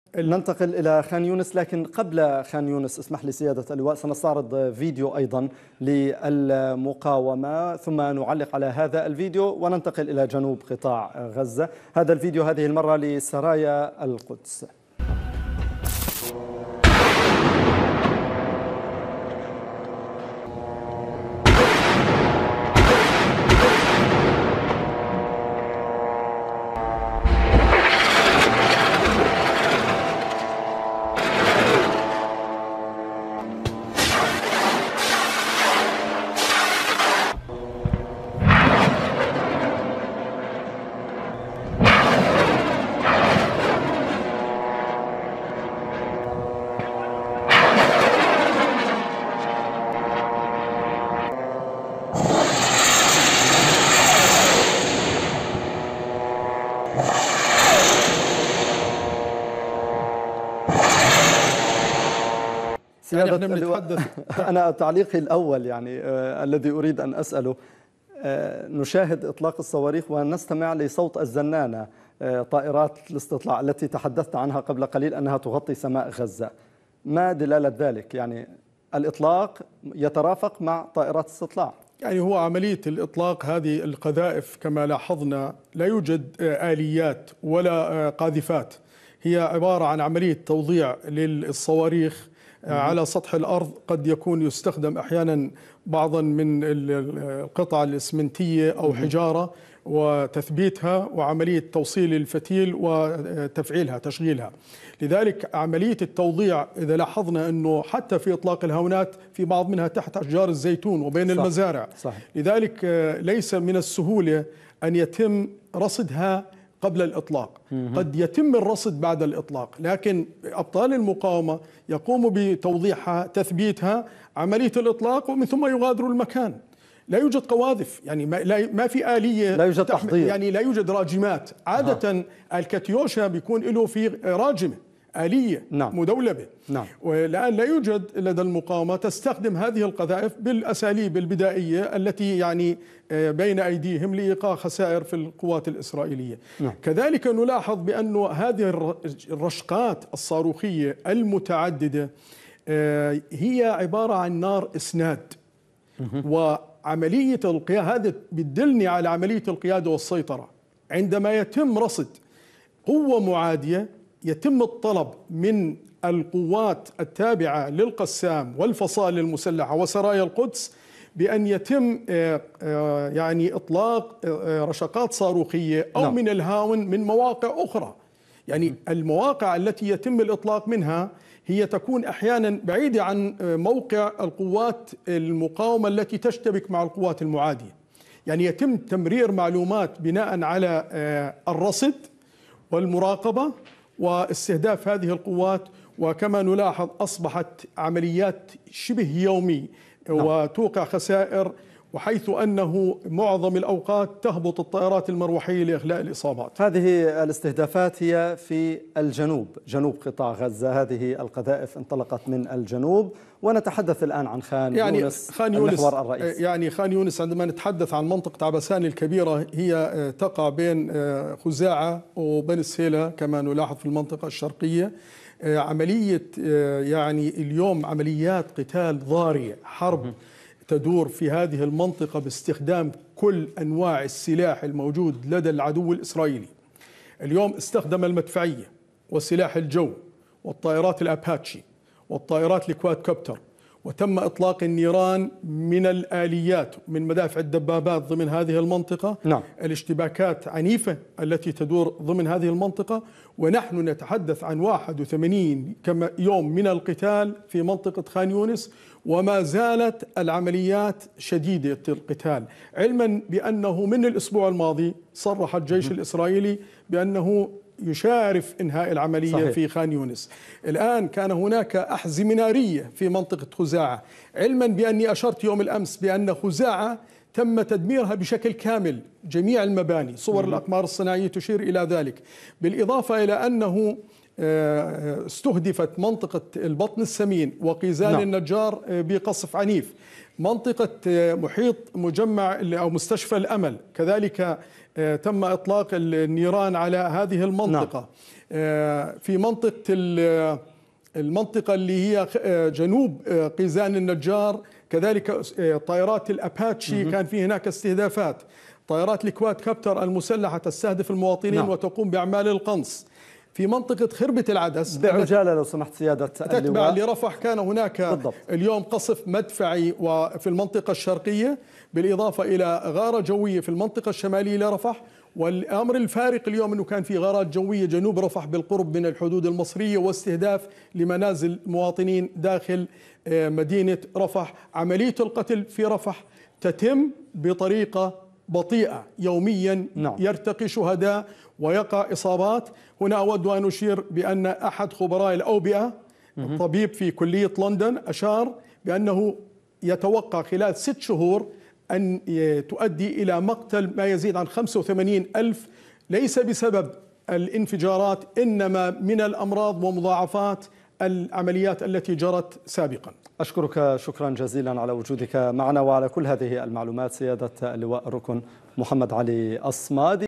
المقاومة توقع بتحشدات الاحتلال خسائر ب"الهاون"وطائرات الاستطلاع الاسرائيلية تفشل في كشف مكان الإطلاق
مشهد لسرايا القدس رافقه صوت طائرة استطلاع إسرائيلية و تحليل للخبير و المحلل العسكري.